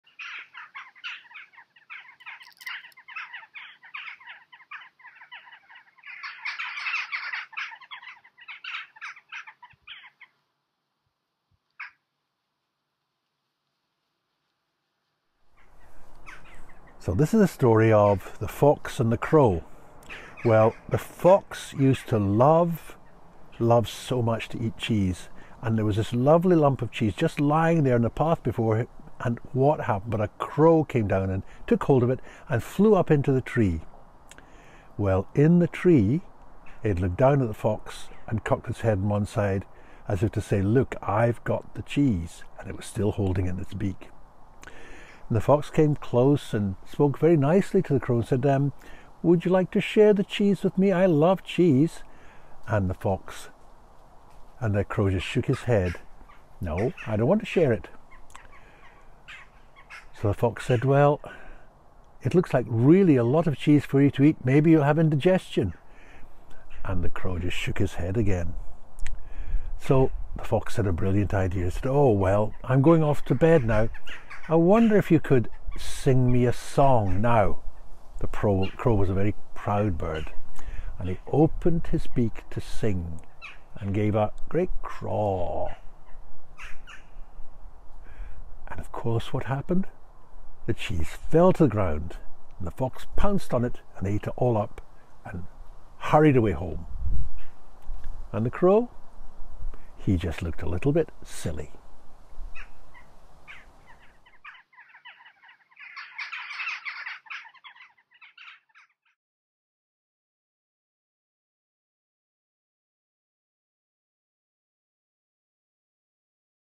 17 The fox and the crow
This video is about a clever fox and greedy crow.